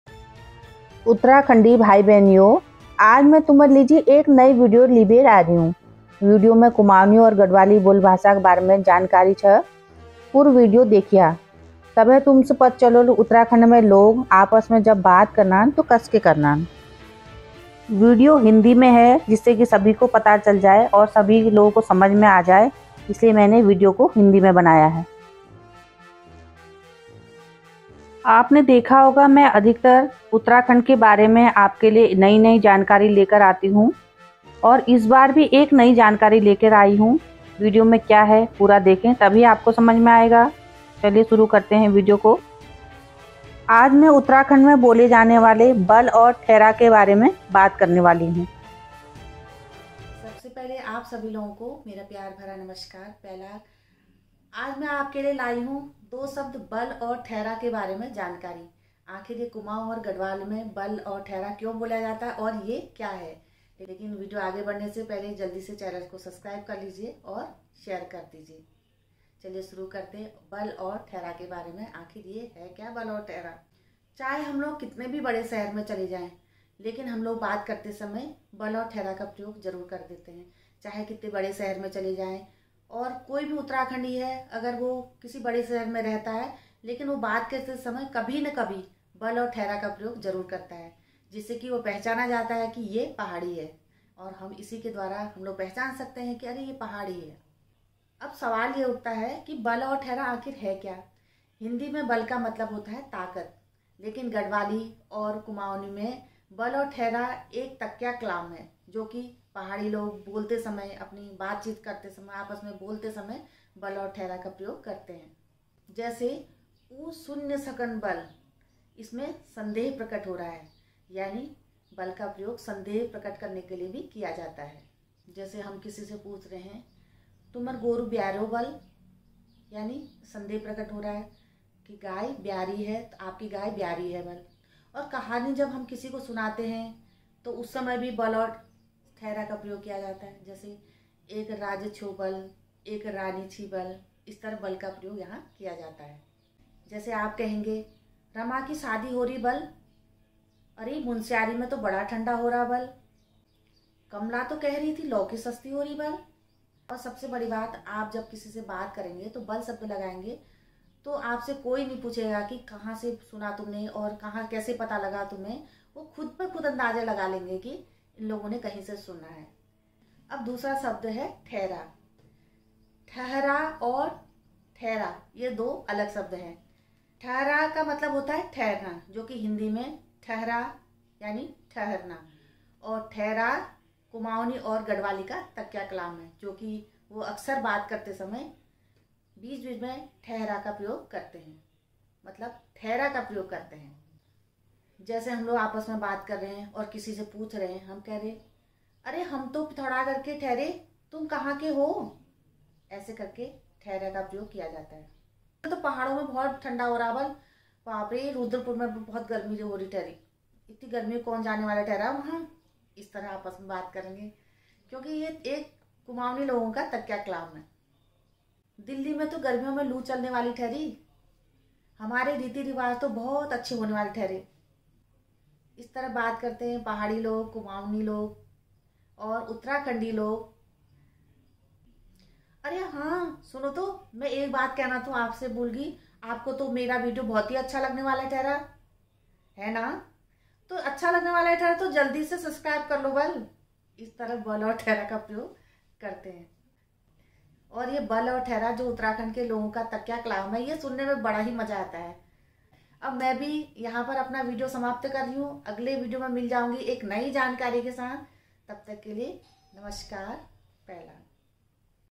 kumaoni language/पहाड़ी बात करते समय बल और ठेरा क्यों बोलते हैं/pahadilanguage
आप सभी को मेरा प्यार भरा नमस्कार पैलाग।।
आज फिर से में आपके लिए एक नया विडियो लाई हूं।
इस विडियो मे मेने बताया है उत्तराखंड के कुमाऊं मंडल और गड़वाल मंडल में कैसे बात करते हैं और वहा की बोली भाषा क्या है।
विडियो को पूरा देखें 🙏

learnkumaonilaguage